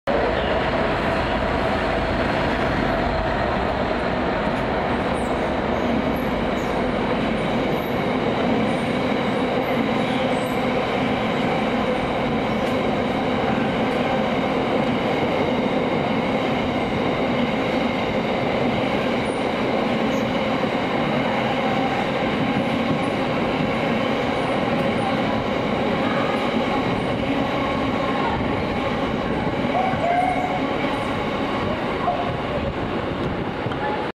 66742 Passes Crewe - 20th September 2013
GBRf's 66742 Passes Crewe working 4F36 Ironbridge - Tuebrook Sidings GBRf Biomass Hoppers.